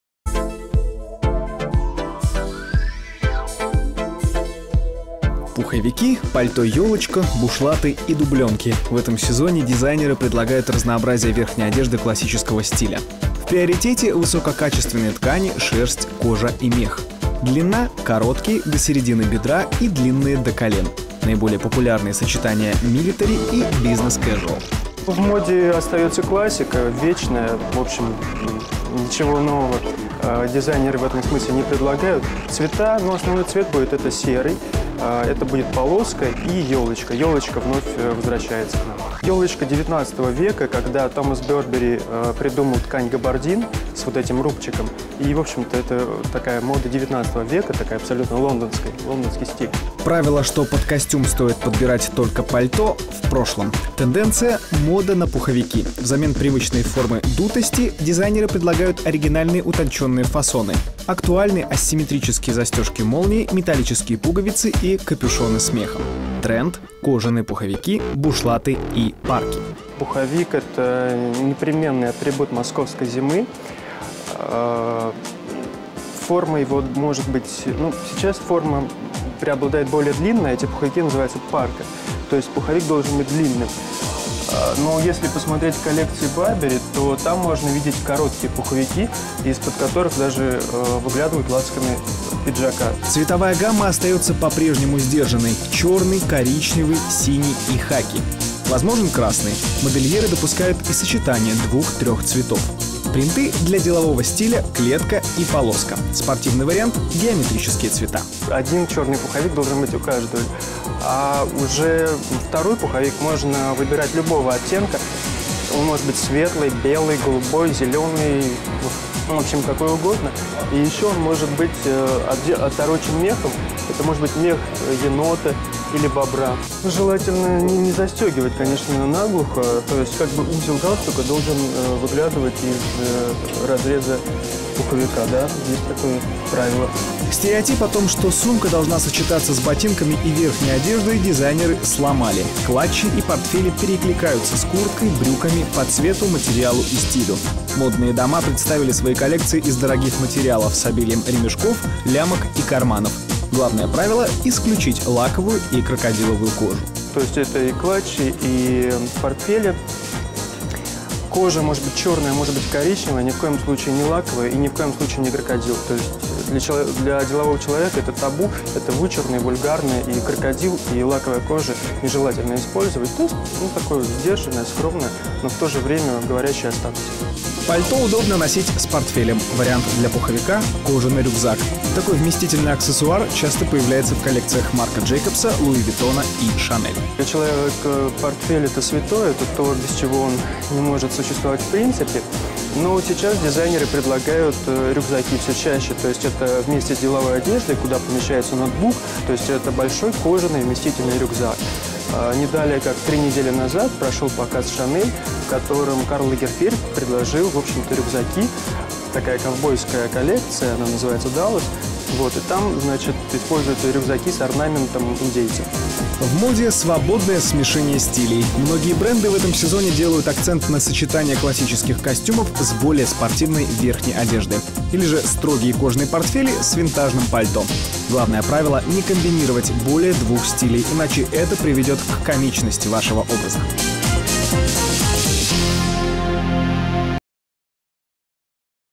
Верхняя одежда классического стиля. Телеканал РБК
Тренды московской зимы. Пуховики, рюкзаки...

Павел Князев, мода,стиль,дизайн,на моде,тв,телеканал,РБК, vogue religion,